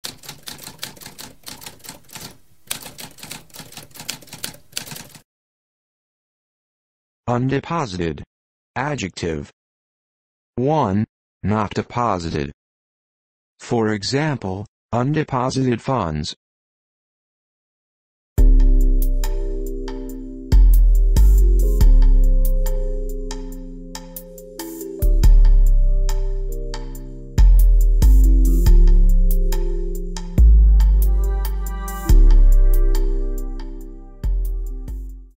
What does undeposited mean?
A spoken definition of undeposited.

Intro Sound:
Typewriter - Tamskp